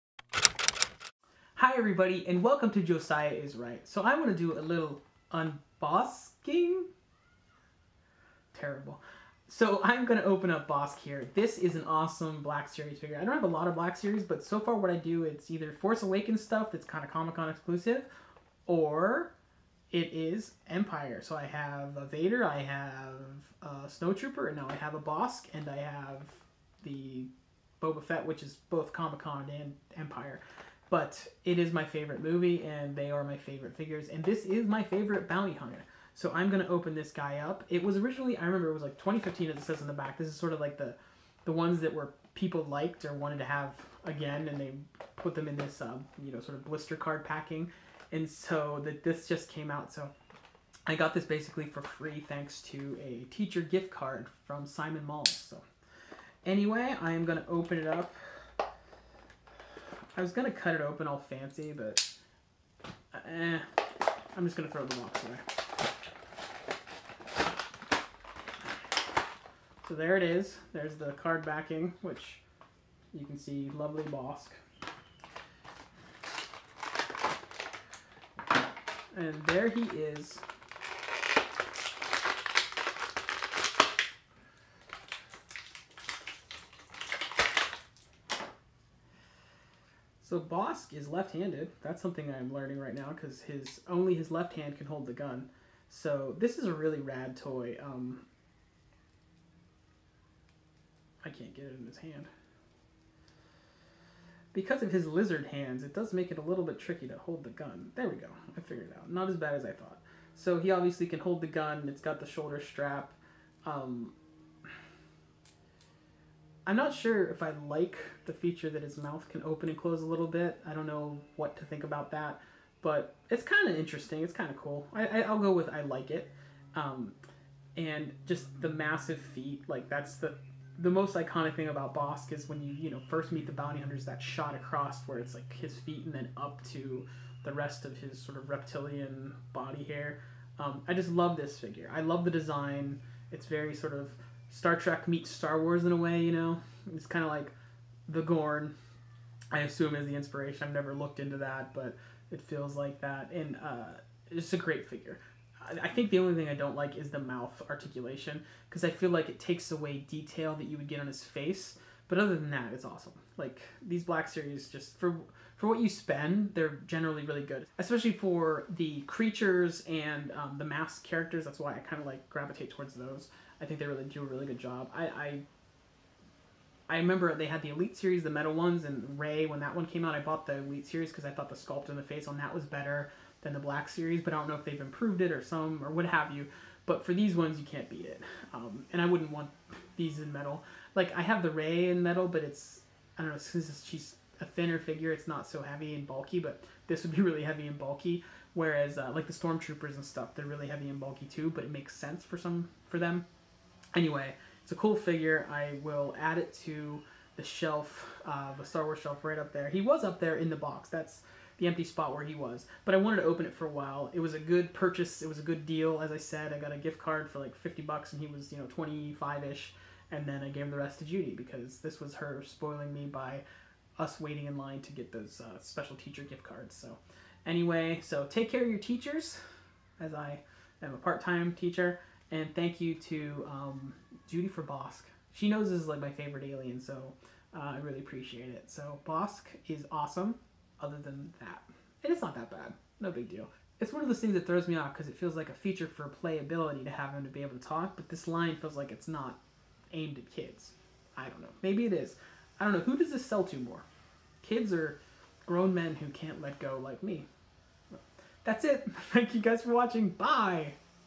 Bossk The Bounty Hunter Unboxing & Action Figure Review - Geek Culture Explained
Geek Culture Explained! Learn How Geek Culture became pop culture and all about this amazing Bossk figure.

Josiah opens up his Black Series Bossk and checks it out.

New to Josiah?